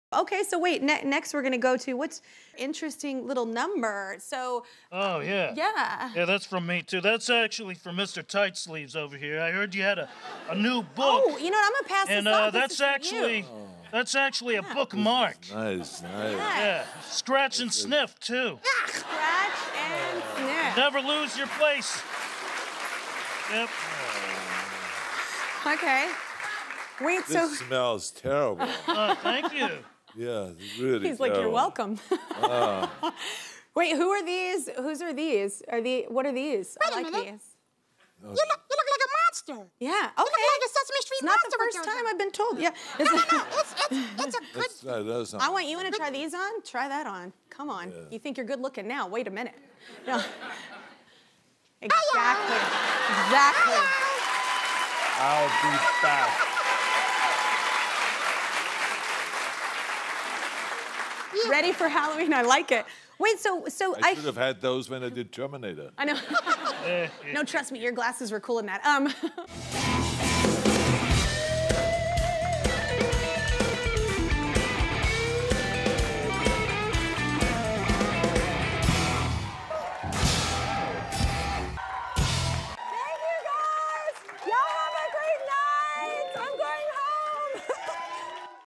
Oscar The Grouch & Elmo Surprise Arnold Schwarzenegger With 'Sesame Street' Gifts
"I'll be back!" Elmo and Oscar the Grouch surprise Arnold Schwarzenegger and Kelly with some special "Sesame Street" gifts, including a scratch and sniff dirty sock bookmark, and a new set of glasses Arnold wishes he had for "The Terminator." Tune in today for more fun with Arnold Schwarzenegger and "Sesame Street."